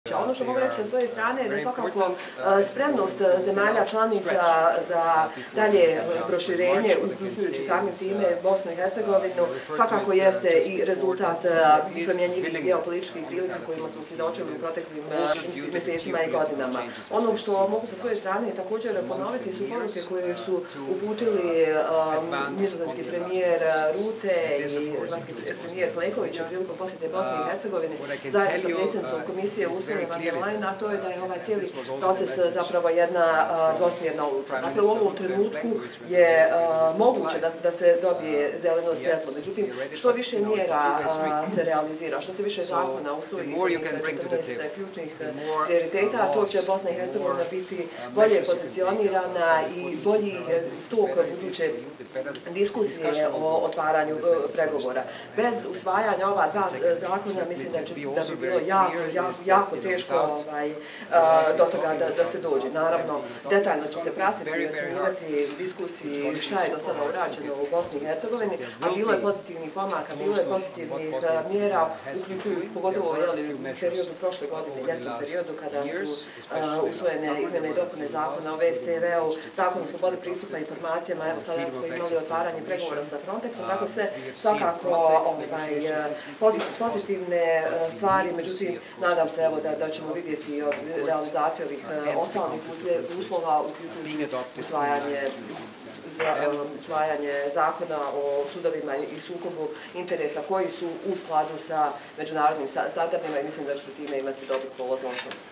Satler: Moguće je da će BiH dobiti zeleno svjetlo za pregovore sa EU
Šef delegacije EU u BiH Johan Satler izjavio je da je bilo pozitivnih pomaka, kada je riječ o putu BiH ka Evropskoj uniji